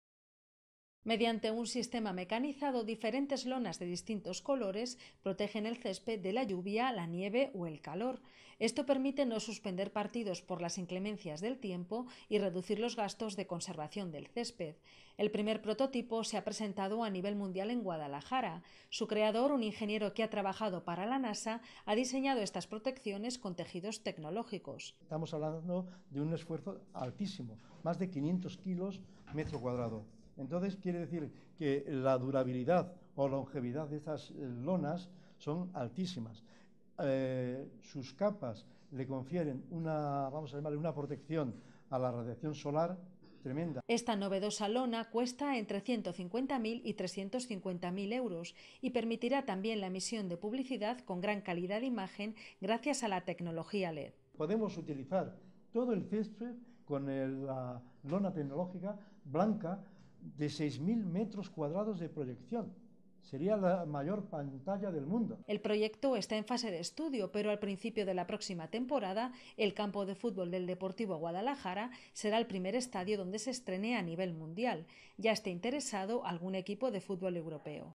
EL PEDRO ESCARTÍN, PRIMER ESTADIO CON LONA TECNOLÓGICA
El campo de futbol de Guadalajara, el Pedro Escartín, será el primero en instalar un novedoso sistema preparado para proteger el césped de inclemencias climatológicas como heladas, lluvia y granizos. Sobre ella, además, se puede proyectar publicidad.